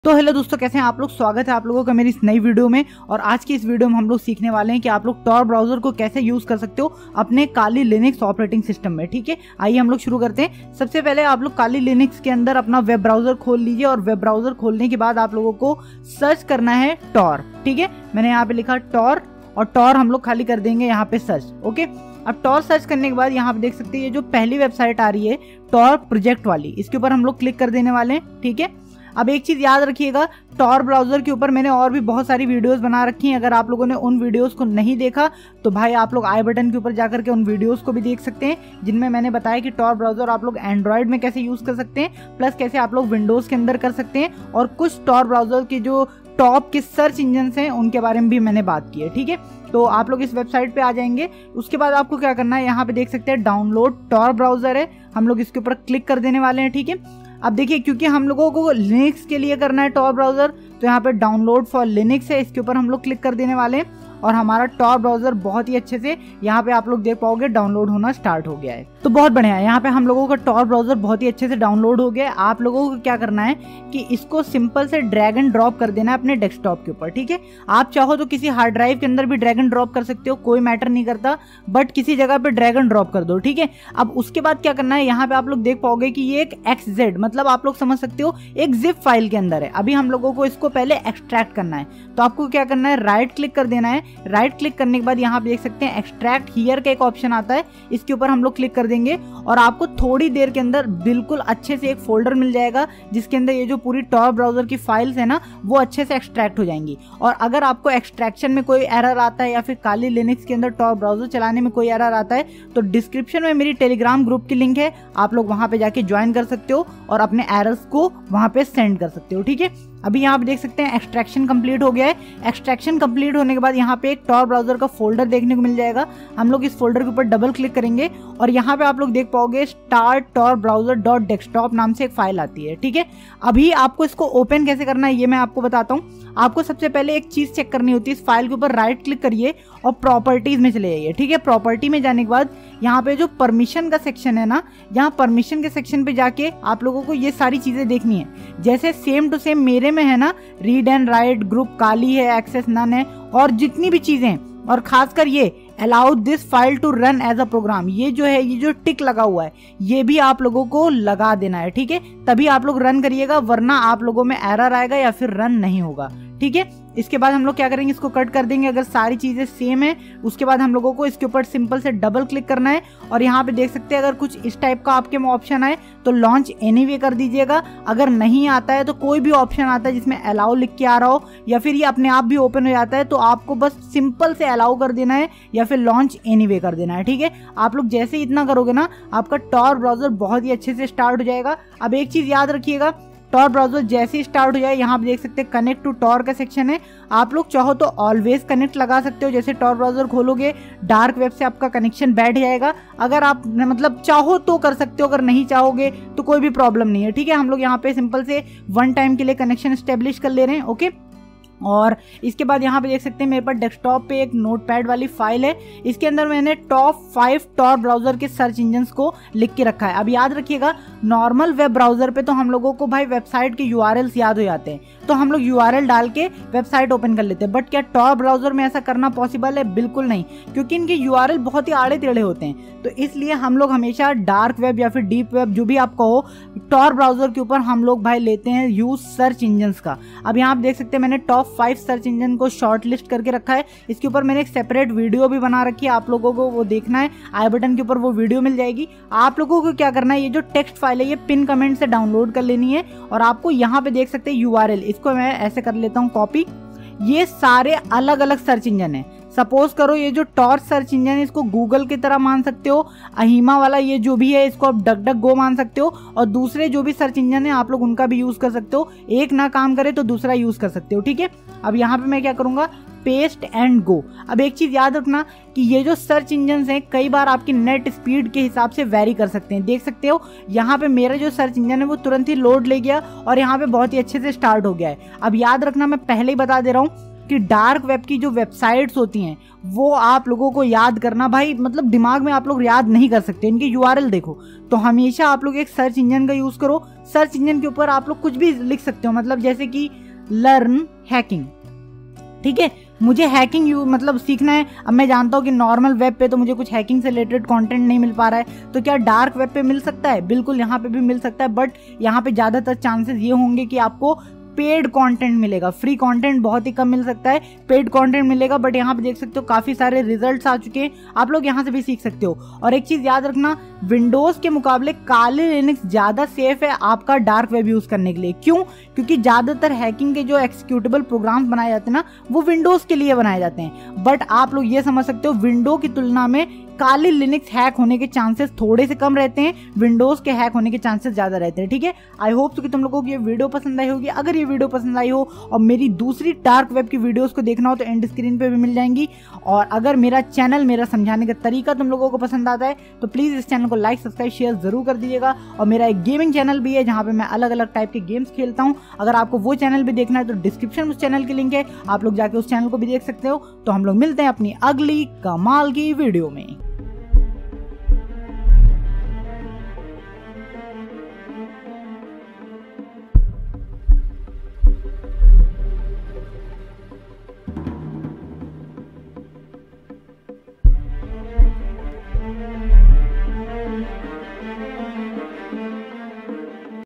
How to use Tor browser in Kali Linux | 2023
About This Video:- Today I will tell u How to use the Tor browser in Kali Linux